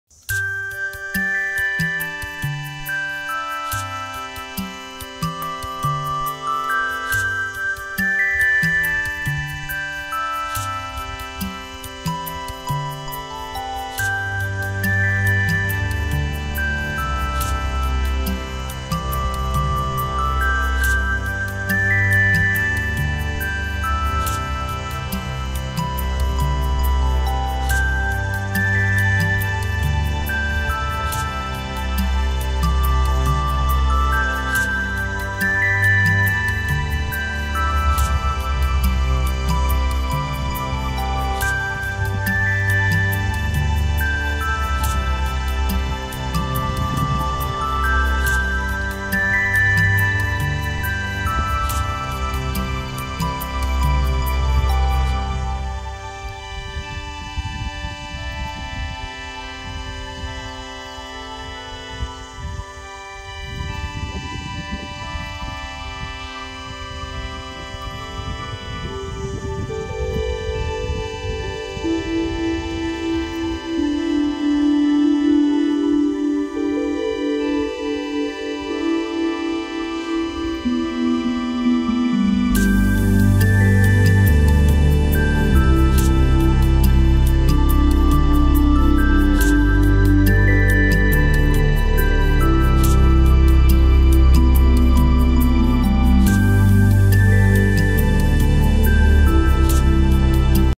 Peacock Eating Tree Leave
Peacock Eating Tree Leave,  Tree is Robinia pseudoacacia